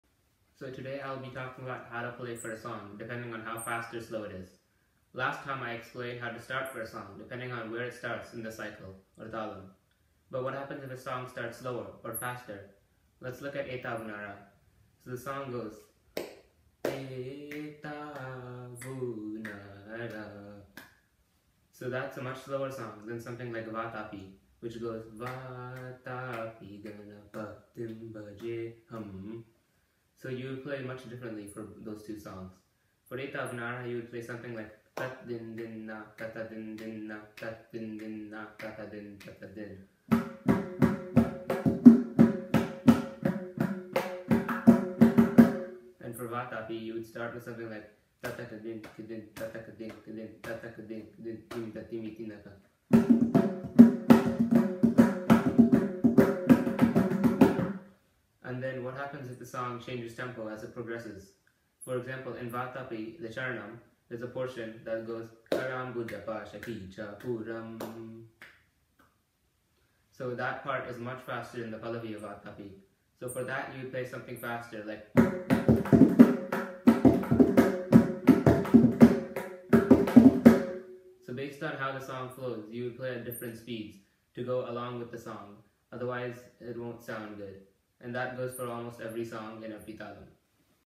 Mridangam 101: Playing for the Tempo of the Song
This video talks about how to play for a song depending on how fast or slow it is.